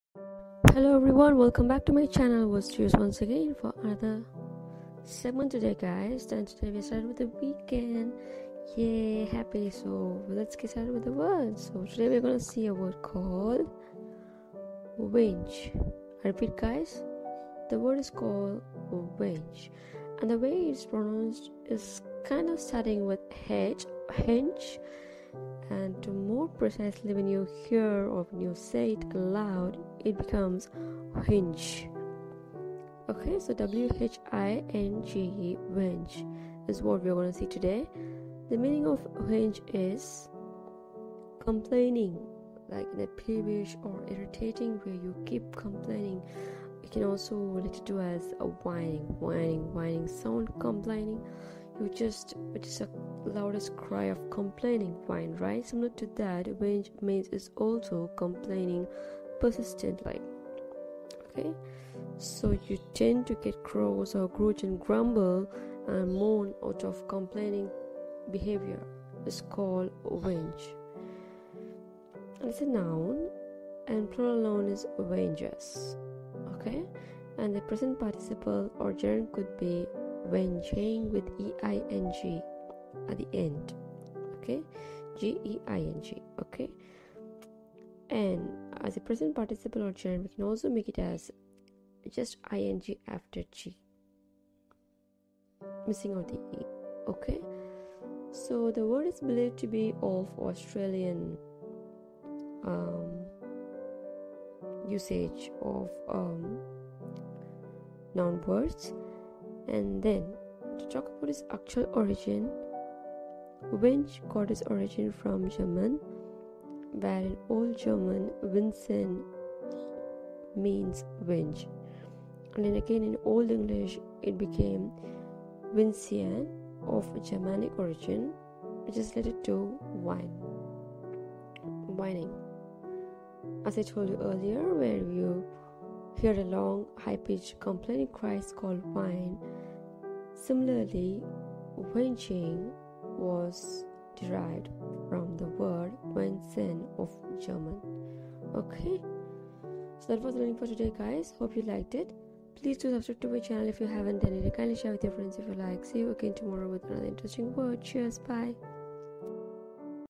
"Whinge" | Word of the Day | Words To Use
This video explains the meaning of the word-Whinge